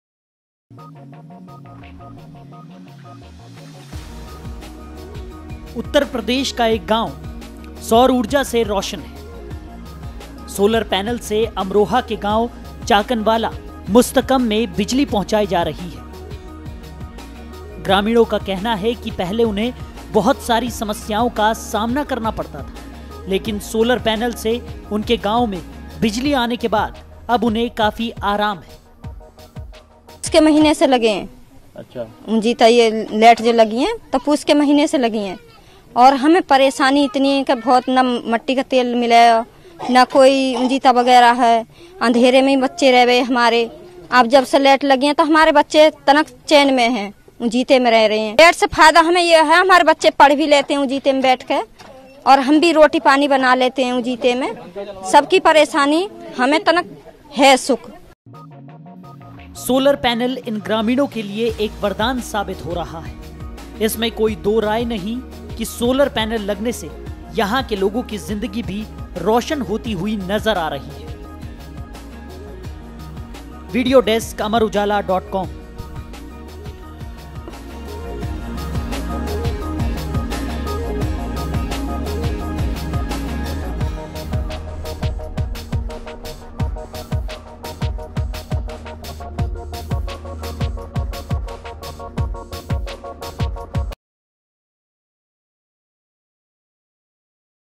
Solar panel से UP के एक village में पहुंचाई जा रही electricity
Uttar-pradesh के एक village में solar panel लगने से गांव वालों को काफी relief मिली है। उनकी life अब रोशन होती हुई नजर आ रही है।